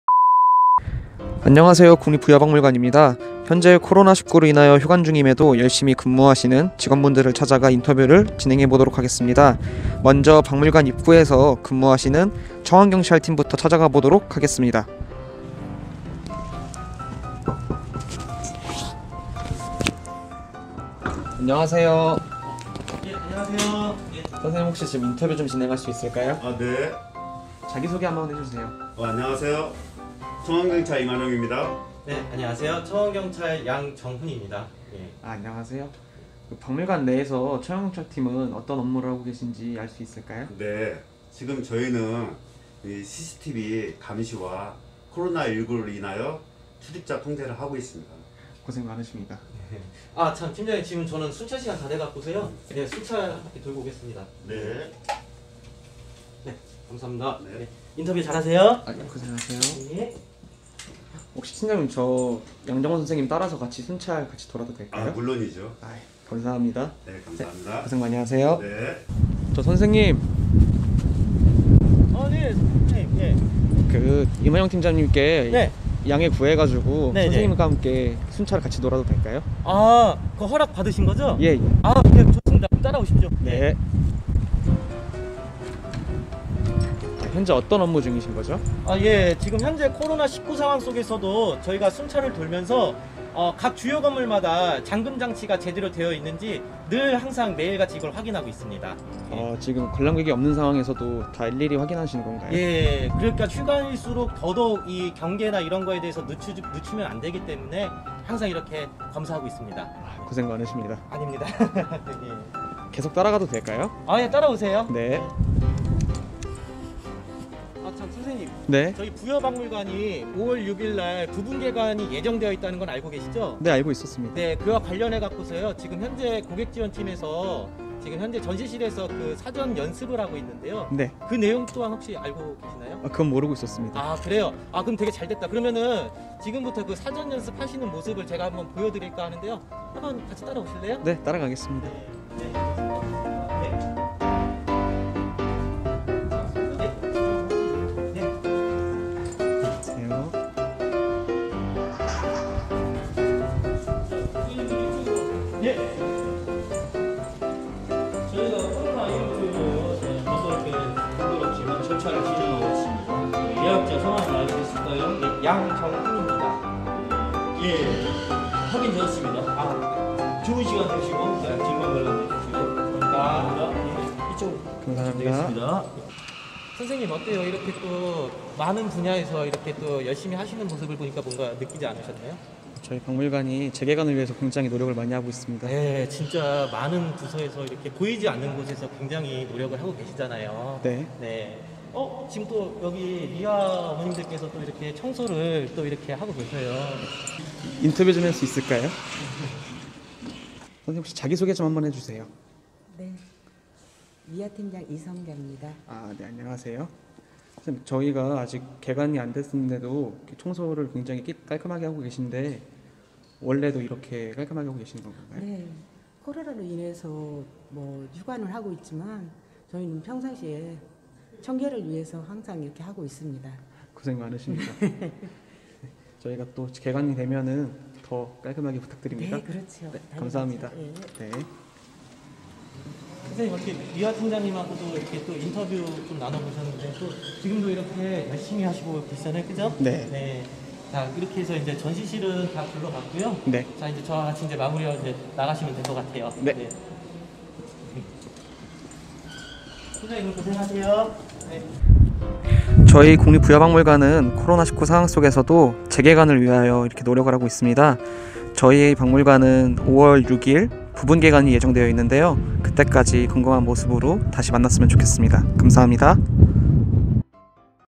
[국립부여박물관] 휴관중에도 출근하세요?_(1편)
여러분 안녕하세요~ 국립부여박물관입니다.^^

코로나19때문에 아직 휴관 중이지만 박물관은 바쁘게 돌아가고 있는데요
그 현장 속으로 한번 함께 가보실까요?